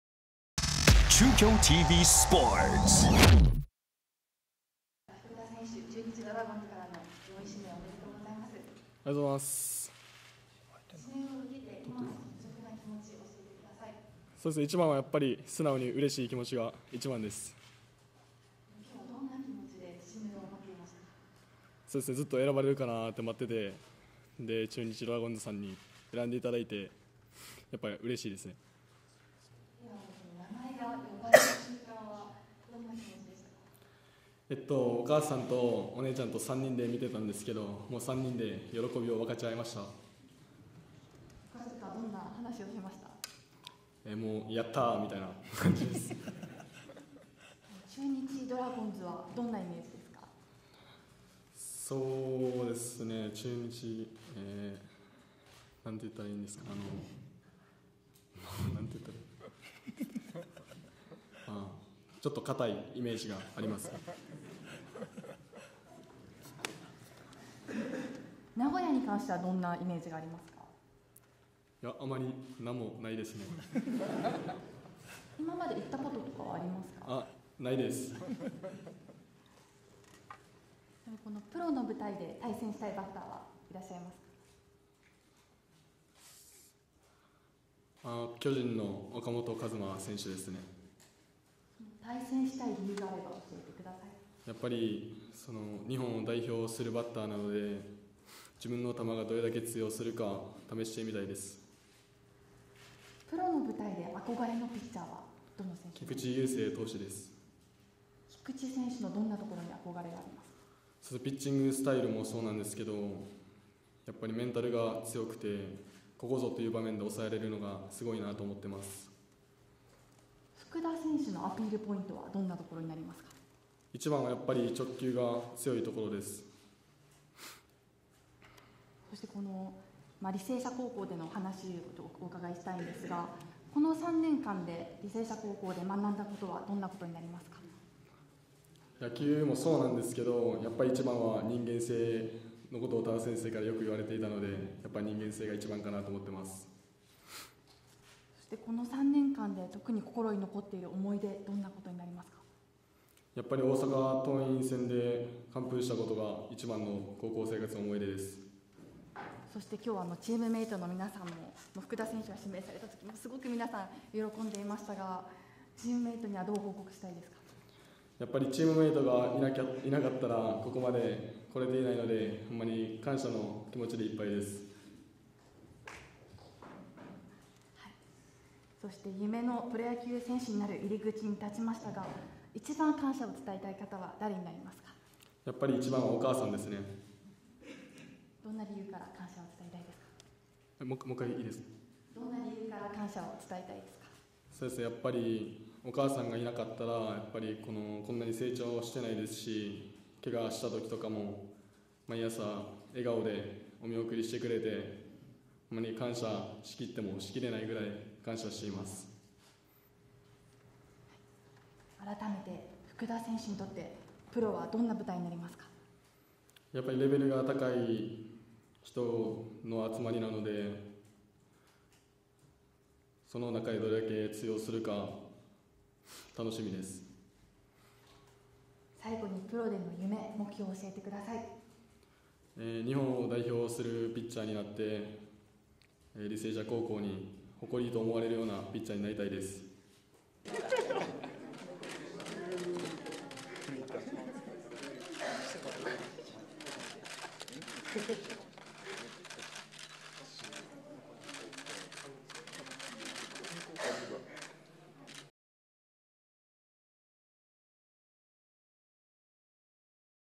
【中日４位指名】履正社 福田幸之介投手 指名会見【ドラフト会議2023】
１０月２６日
中日ドラゴンズは履正社高校 の 福田幸之介投手をドラフト４位で指名。

【東海地方のドラフト候補】